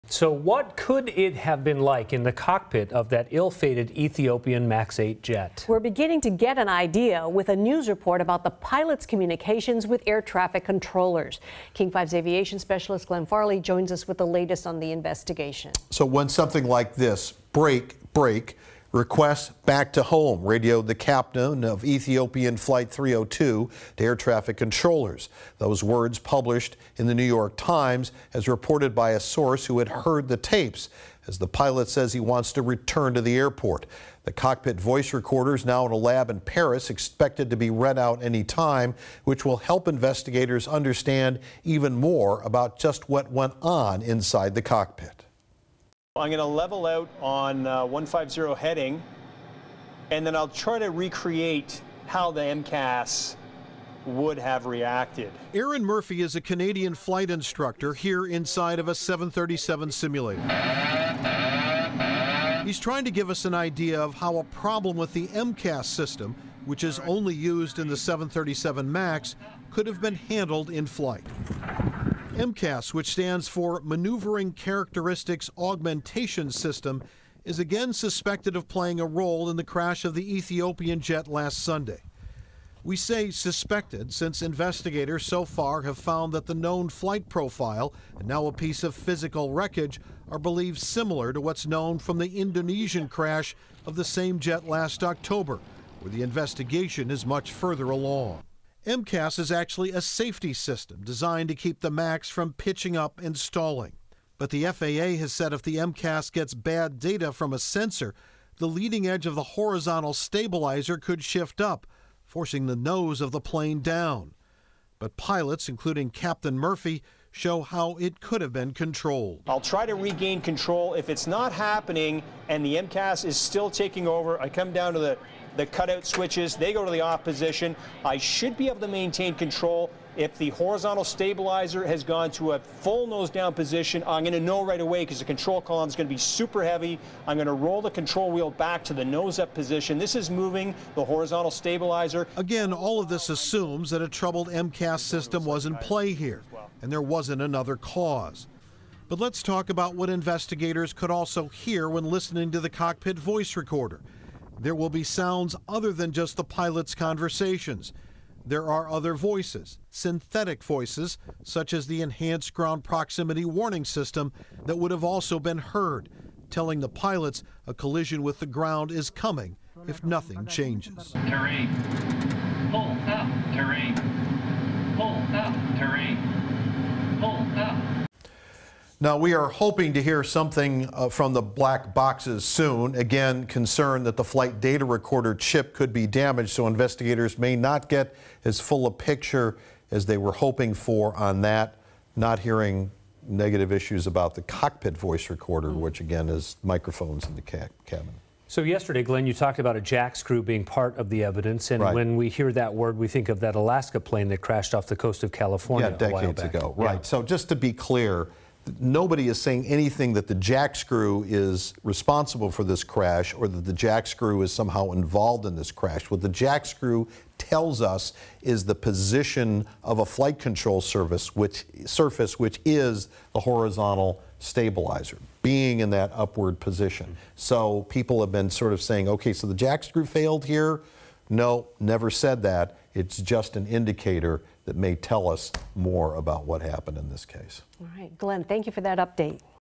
Boeing 737 simulator helps us understand final minutes before deadly crash
A flight instructor in a Boeing 737 simulator shows what it may have been like in the cockpit before the deadly crash of an Ethiopian Airlines Boeing 737 Max on Sunday.